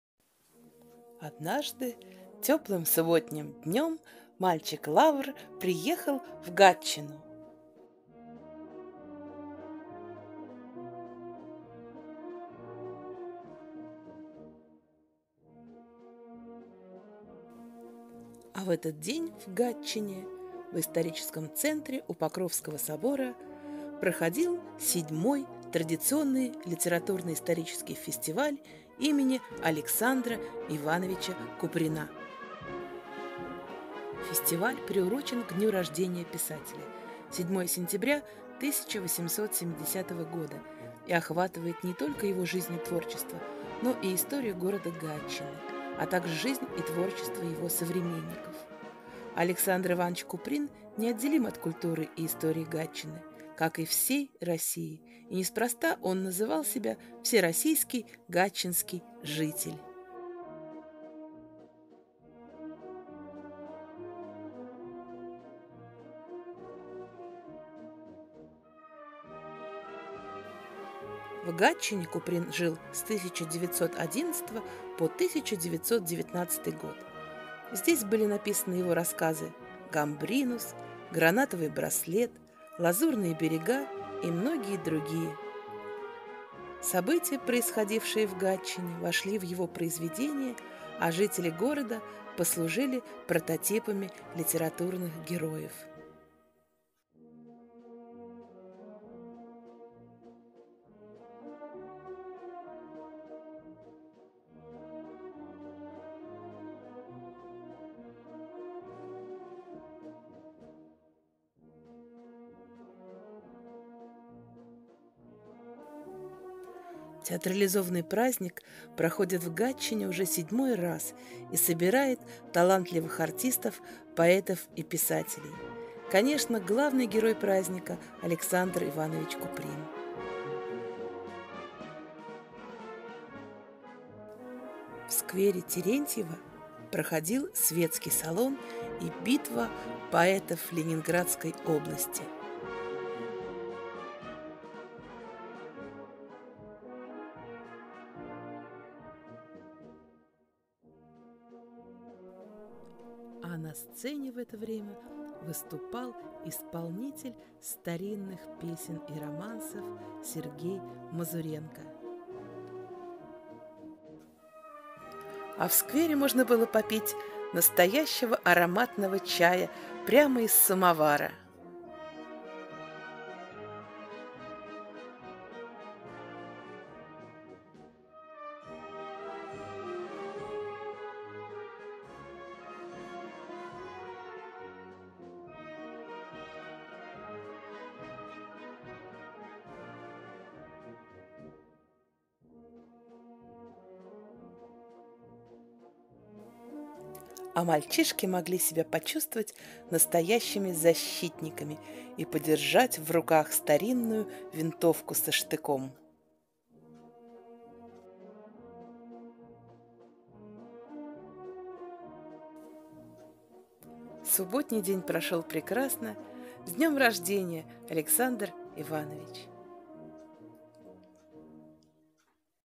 Субботняя прогулка по Гатчине. Купринфест.
Литературно-исторический фестиваль, посвященный Куприну. "Чудная штука эта жизнь"